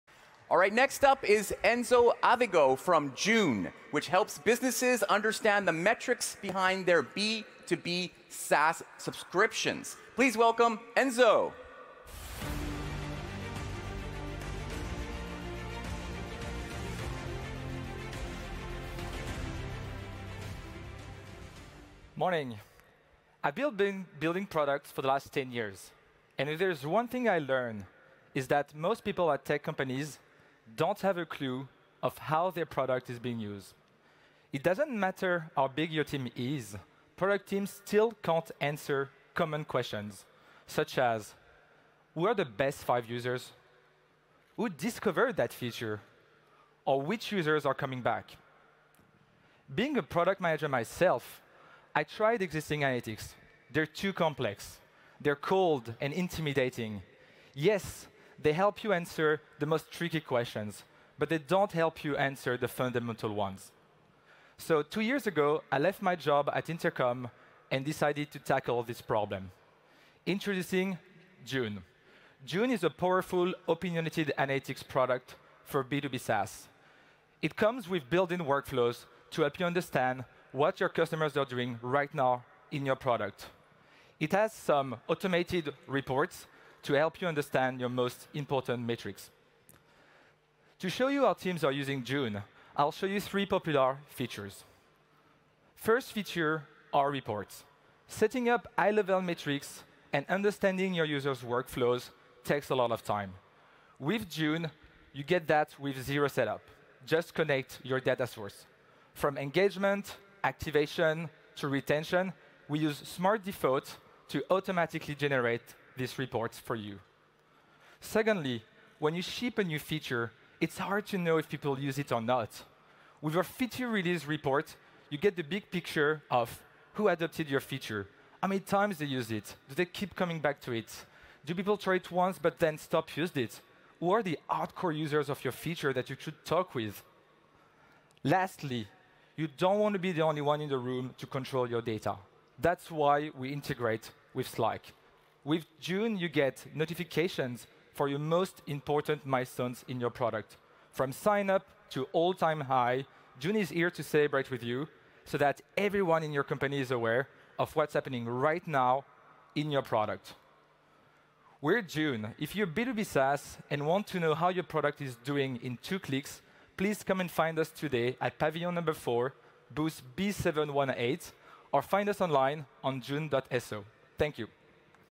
June.so - Breakout Startups
Enzo giving a talk about the future of analytics, at Web Summit 2022